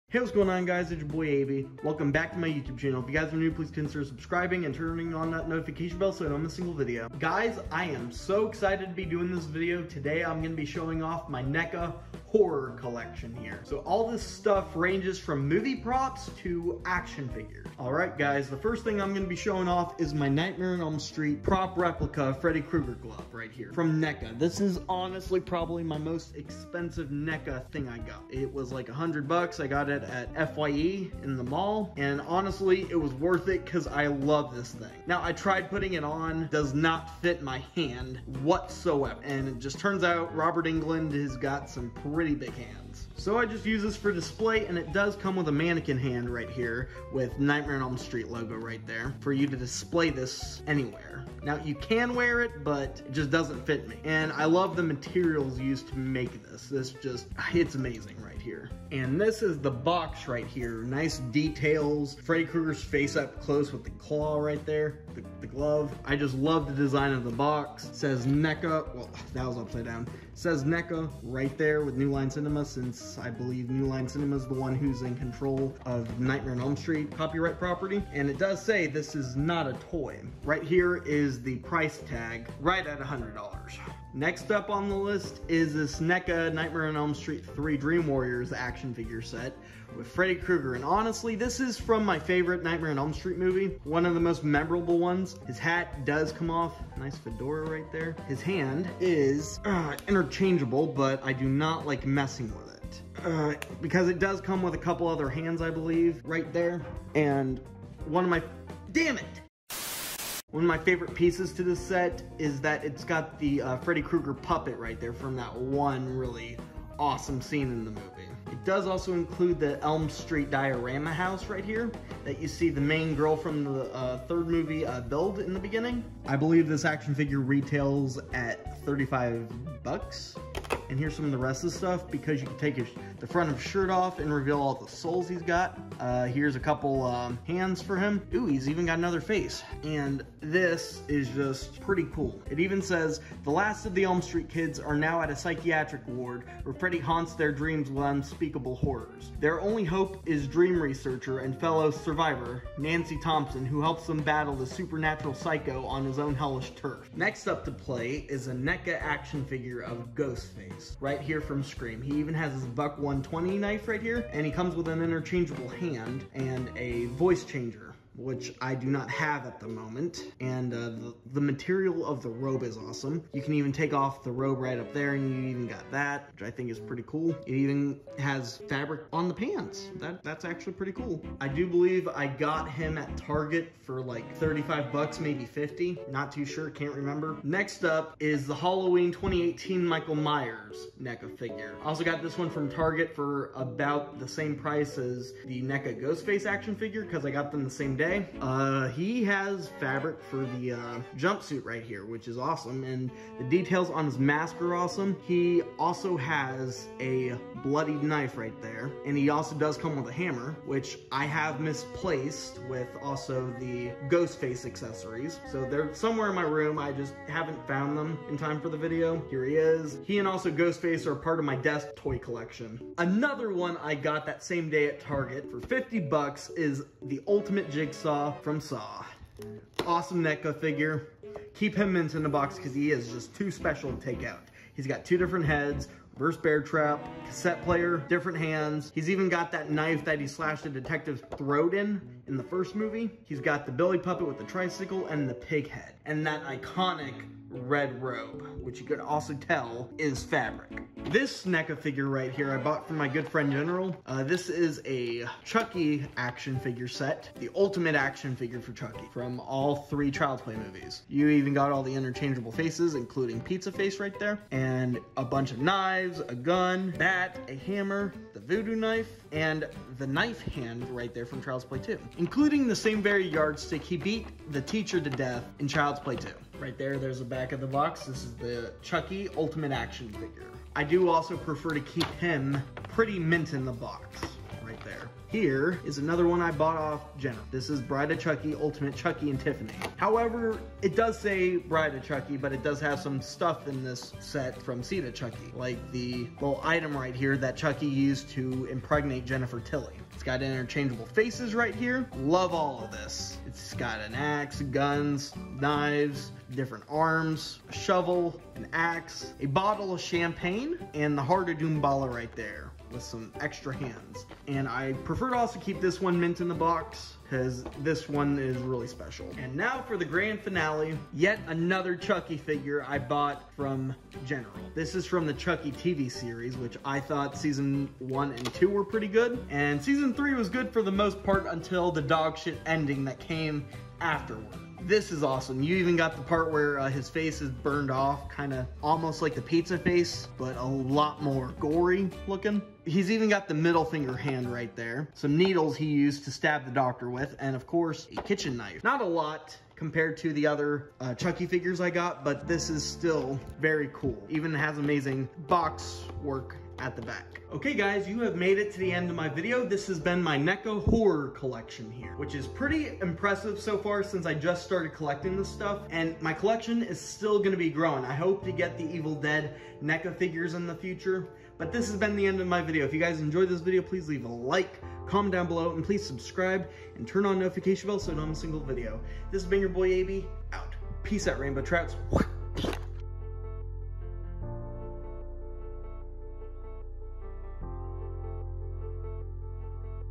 The Dark World of My NECA Horror Collection
Welcome to my NECA Horror Collection of 2024!!

~CHAPTERS~
0:00 - intro
0:11 - my NECA horror collection
0:21 - freddy krueger glove replica
1:24 - freddy krueger dream warriors action figure
2:38 - ghostface action figure
3:08 - michael myers action figure
3:44 - the ultimate jigsaw action figure
4:11 - all my NECA chucky figures
6:00 - outro